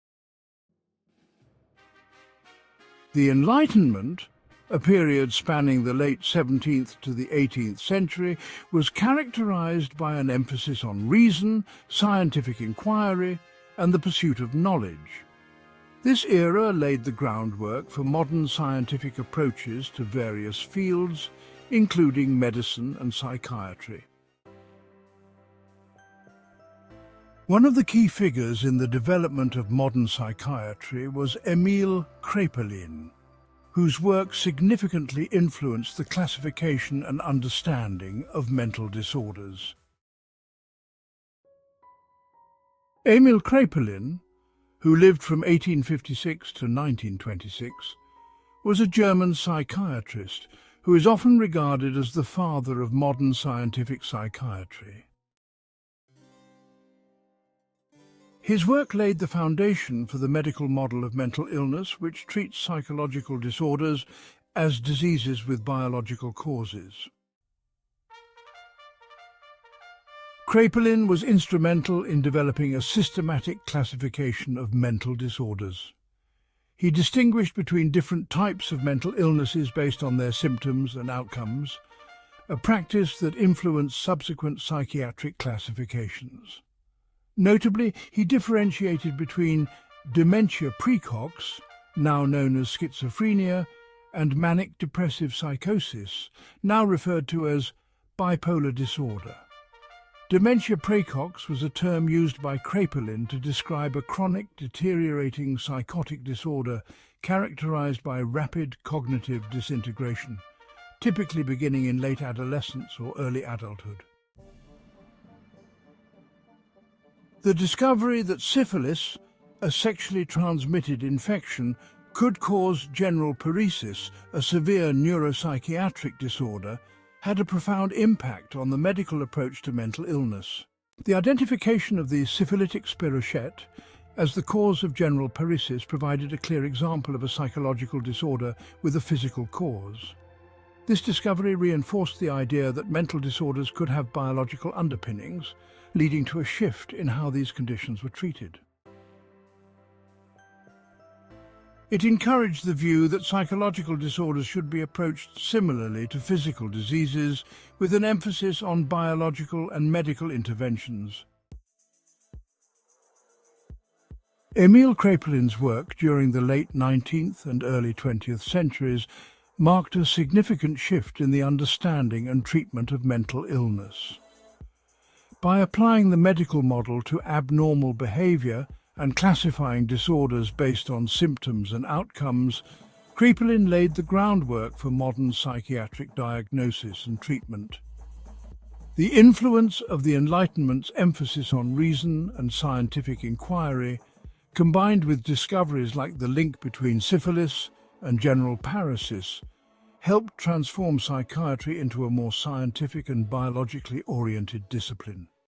The Enlightenment's Influence on Modern Psychiatry: Emil Kraepelin's Legacy
Dive into the transformative era of the Enlightenment and discover how it paved the way for modern psychiatry through the groundbreaking work of Emil Kraepelin. This video explores how the Enlightenment's emphasis on reason and scientific inquiry influenced the medical model of mental illness, leading to a more systematic and biological approach to understanding psychological disorders.
Learn about Kraepelin's pioneering efforts in classifying mental disorders, including his identification of Dementia Praecox (now known as schizophrenia) and Manic-Depressive Psychosis (bipolar disorder). We also examine the impact of the discovery that syphilis could cause general paresis, highlighting how this finding reinforced the view of psychological disorders as diseases with physical causes.
Join us as we unravel the historical developments that shifted mental health treatment from superstition to science, setting the stage for contemporary psychiatric practices. Perfect for history buffs and psychology enthusiasts, this video offers a fascinating look at the evolution of mental health care.